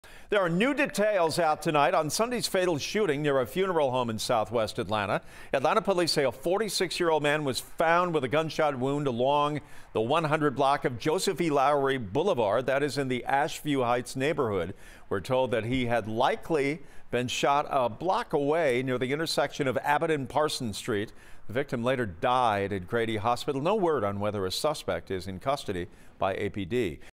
Man dies after being shot in Ashview Heights near funeral home
Officers were approached by people that were telling them that another person had been shot inside a vehicle, according to a statement.